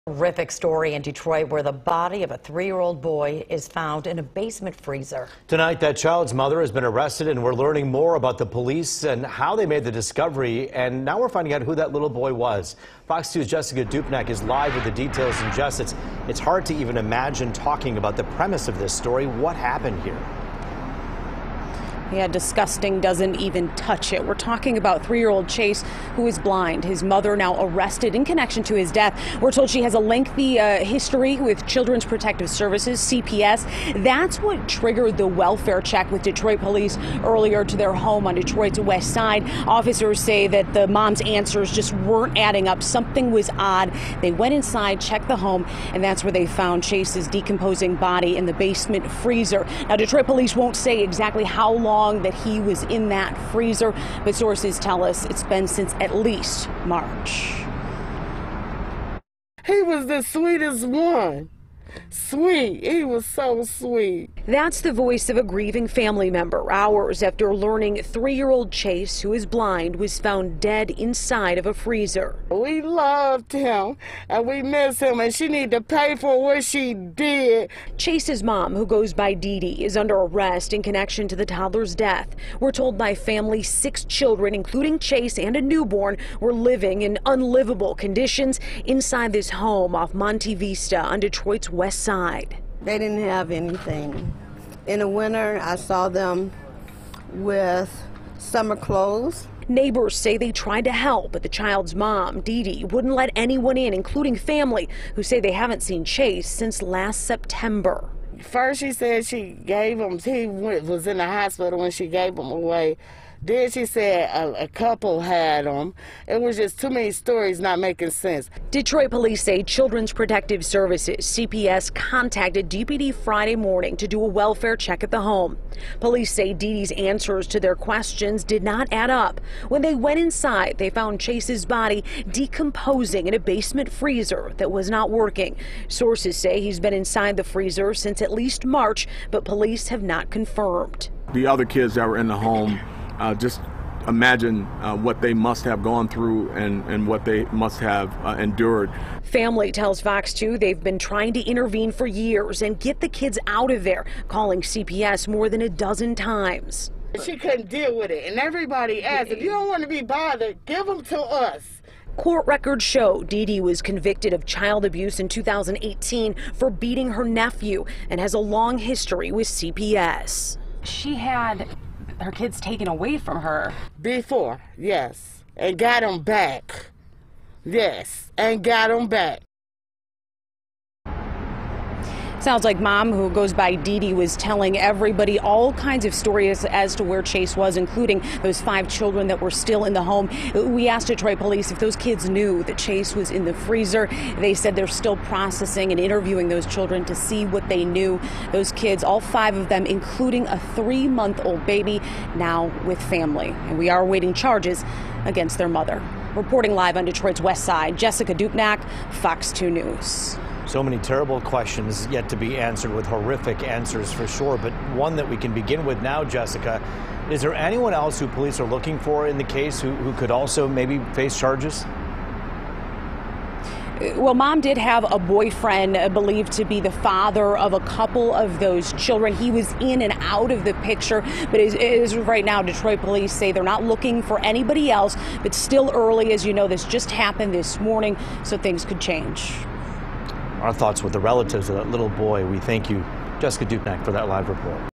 3-year-old Detroit boy found dead in freezer
The body of a 3-year-old boy named Chase was found in a freezer in Detroit. Neighbors said they tried to help the mother, who also had other children living at the home, but she refused it.